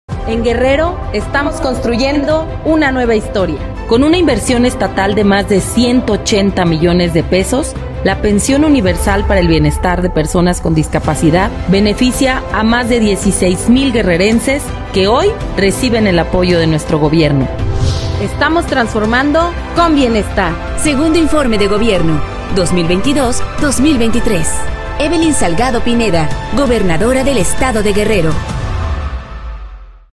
En Guerrero la justicia social es una realidad de la mano de la gobernadora Evelyn Salgado
En Guerrero la justicia social es una realidad de la mano de la gobernadora Evelyn Salgado. Hoy más de 16 mil personas con alguna discapacidad reciben pensiones destinadas a sus necesidades. Celebramos un año más transformando Guerrero.